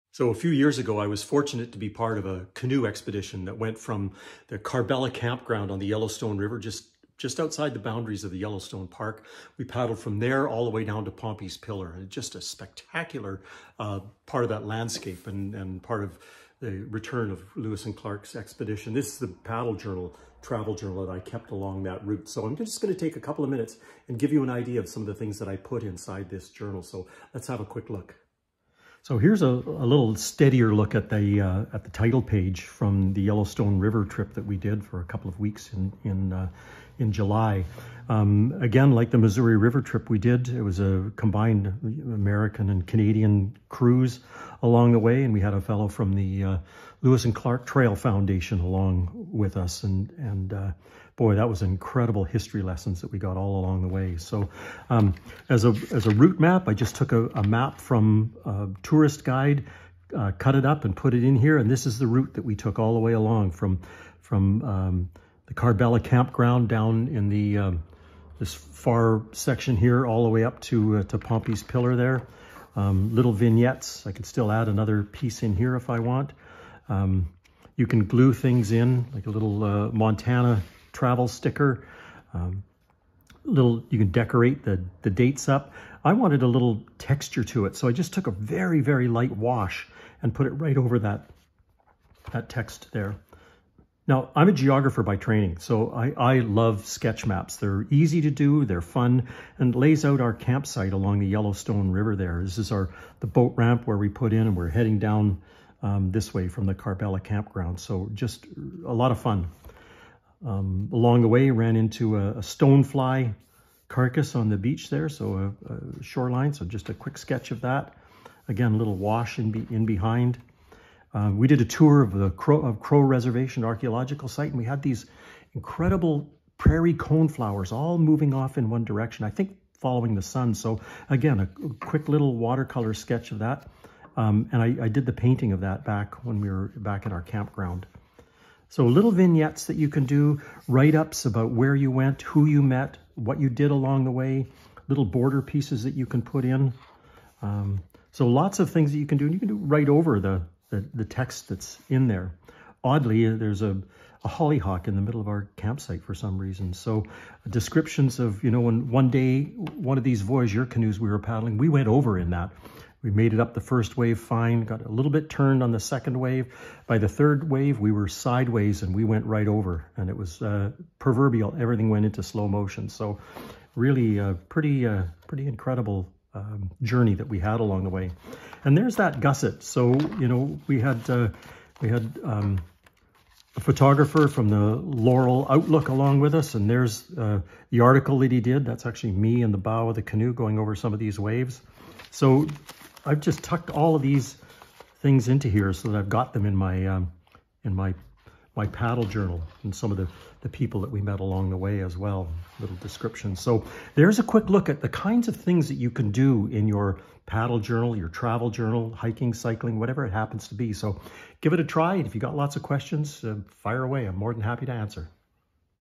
Yellowstone River Paddle Journal
This short video will take you on a quick tour of the Paddle Journal I kept on a 2-week paddle trip along wonderful stretches of the Yellowstone River in Montana.